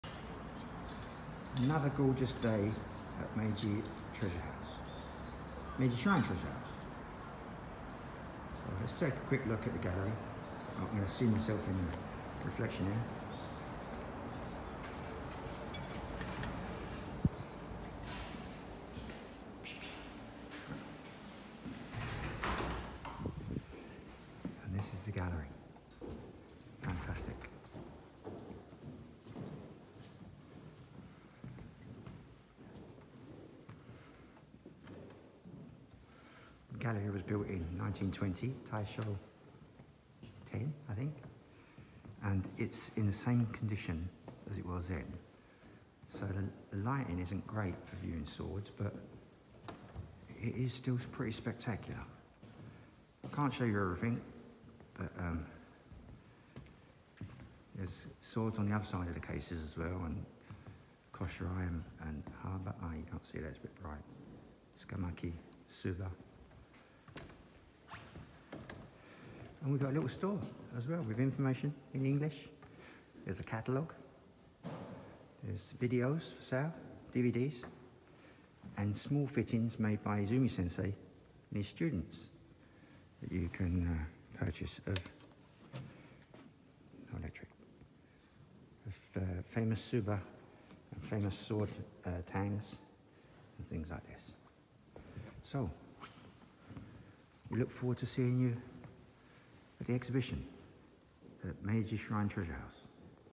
NBSK Sword Craftsmen’s Exhibition at Meiji Shrine Treasure Hall November 2022
The Society for the Promotion of Japanese Sword Culture held a sword crafts exhibition at Meiji Shrine Treasure Hall in November 2022. There was also a forging demonstration by Gassan Sadatoshi sensei and his son, Sadanobu, and their students. On until November 20.